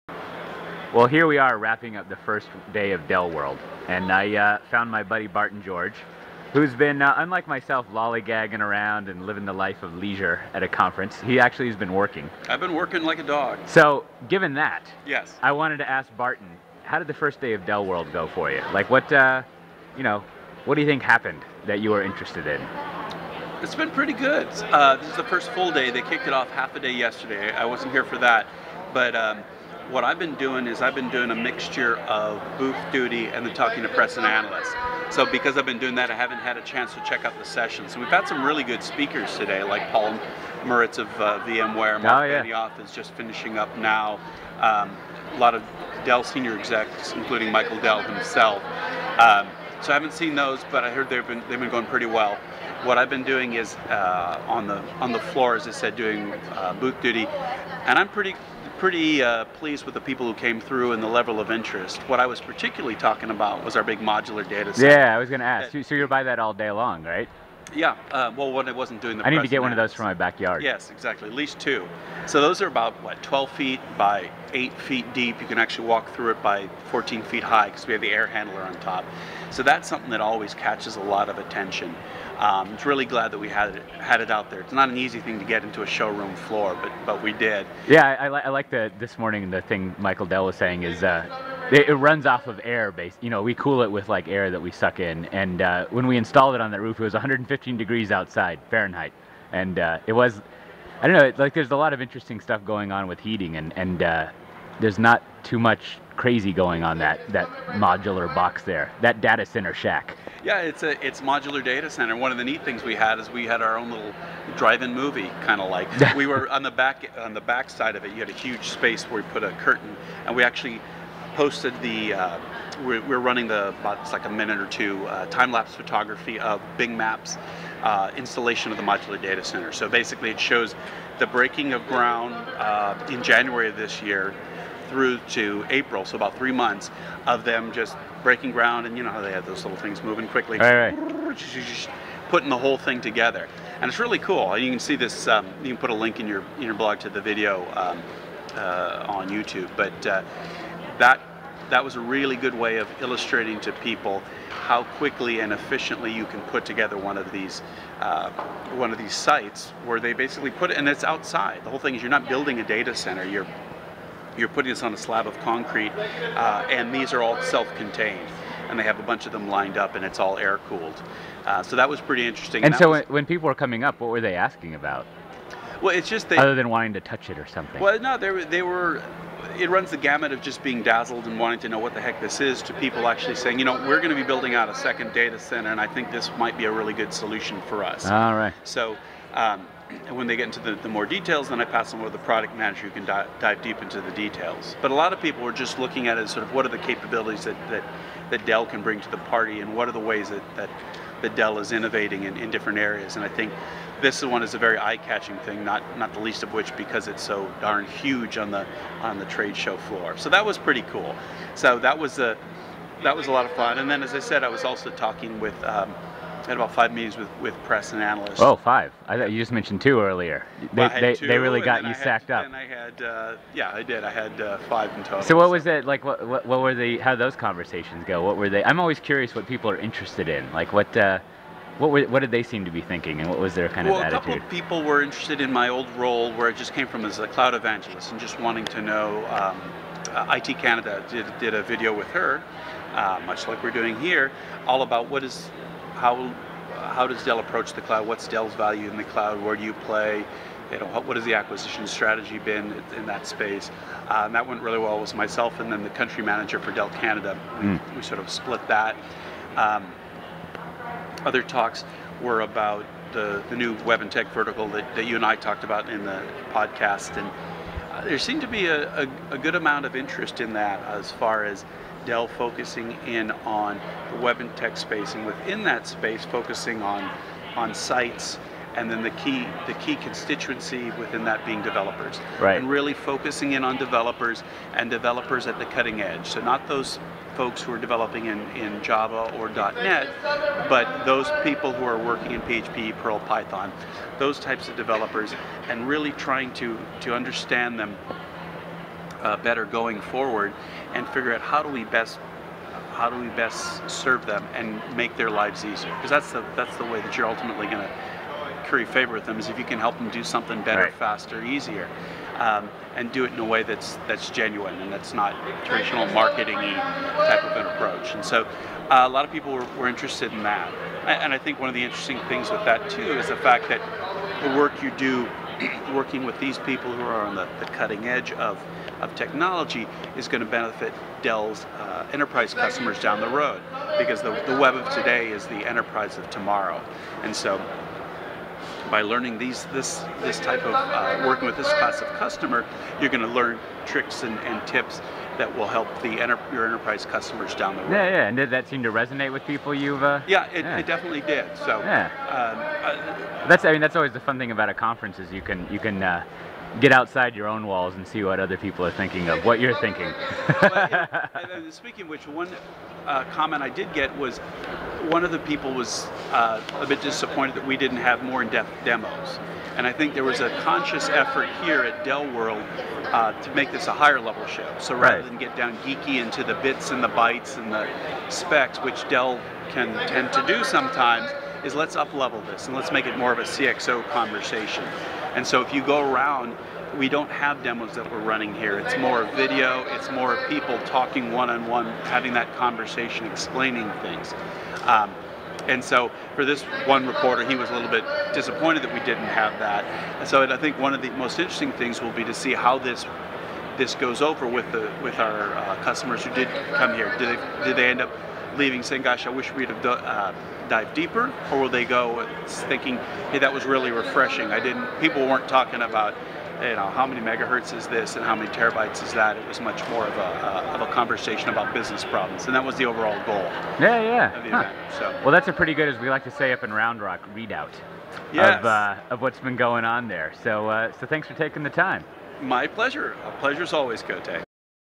Barton George on the first day of DellWorld
After the first day of DellWorld, I ask Barton George how it's been going. He talks about the modular datacenter and other conversations he'd been having.